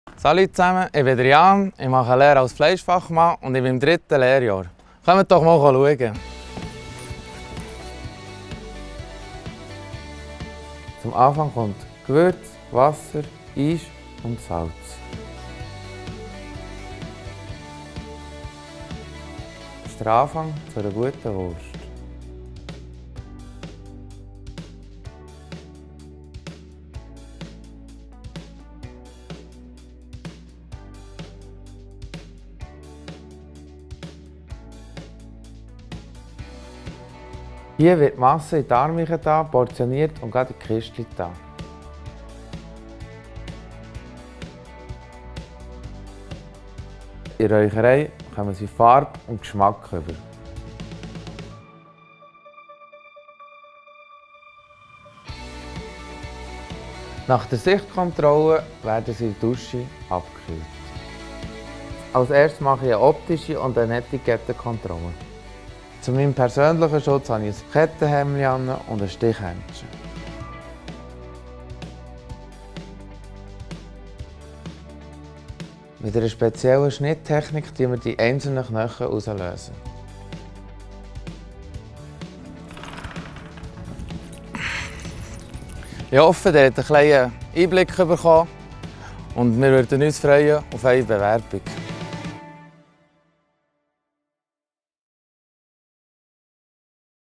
Lehre als Fleischfachmann/-frau bei der Bigler AG
Wir bieten eine abwechslungsreiche und spannende Lehrstelle als Fleischfachmann/-frau EFZ. Wenn dich dieser Lehrberuf interessiert, dann bist du bei uns, Bigler, genau richtig. Wir suchen motivierte und begeisterte Talente. Starte deine Zukunft bei uns und bewirb dich noch heute.